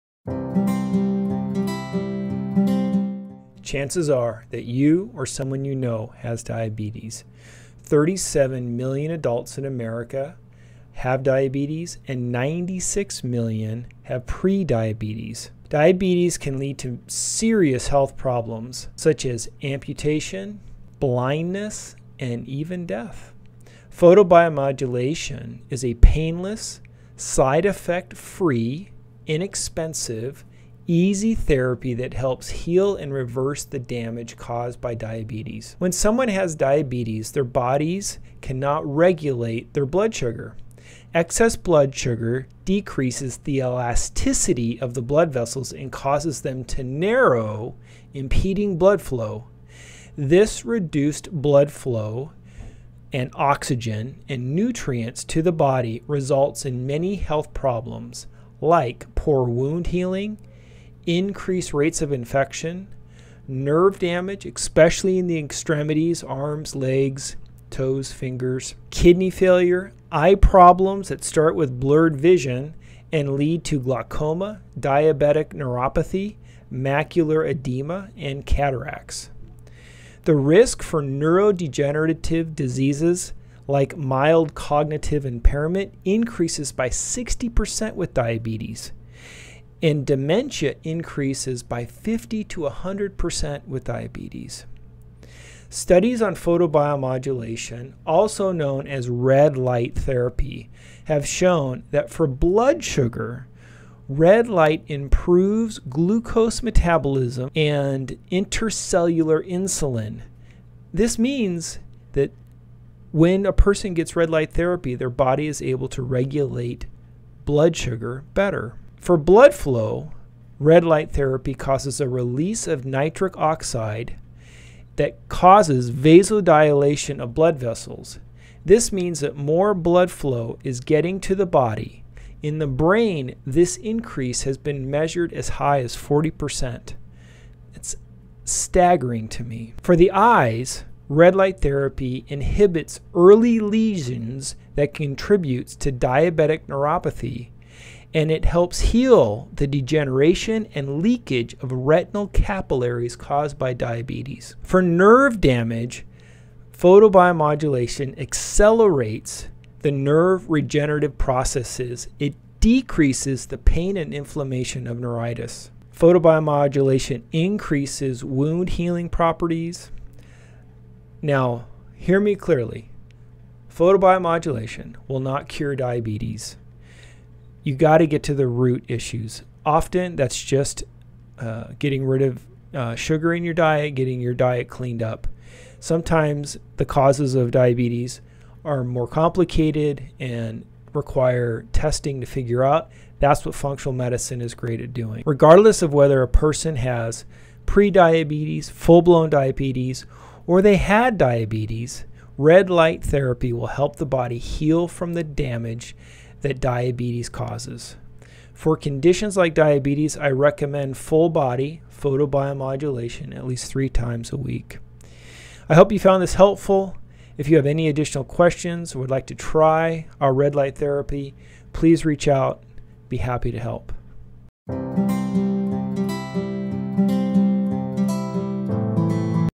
Red Light and Diabetes
Video Information:
37 million adults in America have diabetes and 96 million have prediabetes

Diabetes is a major health problem that can lead to many serious health problems including amputation, blindness and death.

Photobiomodulation, also known as Red Light therapy, is a painless, side effect free, inexpensive, easy therapy that helps heal and reverse the damage caused by diabetes.

When someone has diabetes their body cannot keep their regulate their blood sugar. Excess blood sugar decreases the elasticity of blood vessels and causes them to narrow, impeding blood flow. This leads to a reduced blood flow, oxygen and nutrients to the body and results in many health problems like:
o Poor wound healing
o Increased rate of infections
o Nerve damage in the legs, feet, and toes, arms, hands, and fingers
o Kidney failure
o Eye problems start with blurred vision and lead to glaucoma, diabetic retinopathy, macular edema and cataracts
o The risk for neurodegenerative diseases like mild cognitive impairment increases up to 60% and dementia 50 to 100% with diabetes.

Studies on Photobiomodulation aka Red light have shown that:
o For blood sugar PBM improves glucose metabolism and intracellular insulin. This means that it helps the body regulate sugar.
o For blood flow Red Light causes a release of nitric oxide from cells the triggers vasodilation of blood vessels. This means that blood vessels get bigger and that increases blood flow delivering oxygen and nutrients to the body. In the brain this increase was measured to by a 40% increase.
o For the eyes RLT Inhibits Early Lesions That Contribute to Diabetic Retinopathy and helps heal the degeneration and leakage of retinal capillaries caused by diabetes.
o For nerve damage PBM accelerates the process of nerve regeneration and decreases the pain and inflammation of neuritis.
o PBM Improves wound healing properties

Studies mentioned:

Infrared photobiomodulation (PBM) therapy improves glucose metabolism and intracellular insulin pathway in adipose tissue of high-fat fed mice

Low-Intensity Far-Red Light Inhibits Early Lesions That Contribute to Diabetic Retinopathy

Photobiomodulation Inhibits Long-term Structural and Functional Lesions of Diabetic Retinopathy

These results suggest that PBMT can play a very important role in the control of blood glucose levels, and its possible mechanism of action is the induction of greater muscle glycogen synthesis independently of physical exercise.

Healing Effects of Photobiomodulation on Diabetic Wounds PDF : It has been shown to promote tissue regeneration and speed up wound repair by reducing inflammation and oxidative stress, accelerating cell migration and proliferation, and promoting extracellular matrix production and release of essential growth factors.

The systematic review concludes that PBMT regulates inflammatory cytokines levels, enhances cell proliferation, and migration, thereby improving the wound healing properties.
Effectiveness of Photobiomodulation Therapy on Neuropathic Pain, Nerve Conduction and Plantar Pressure Distribution in Diabetic Peripheral Neuropathy - A Systematic Review : We conclude that photobiomodulation therapy is an effective, non-invasive, and cost efficient means to improve neuropathic pain and altered plantar pressure distribution in diabetic peripheral neuropathy.

Photobiomodulation Therapy (PBMT) in Peripheral Nerve Regeneration: A Systematic Review: PBMT was able to accelerate the process of nerve regeneration, presenting an increase in the number of myelinated fibers and a better lamellar organization of myelin sheath, besides improvement of electrophysiological function, immunoreactivity, high functionality rate, decrease of inflammation, pain, and the facilitation of neural regeneration, release of growth factors, increase of vascular network and collagen.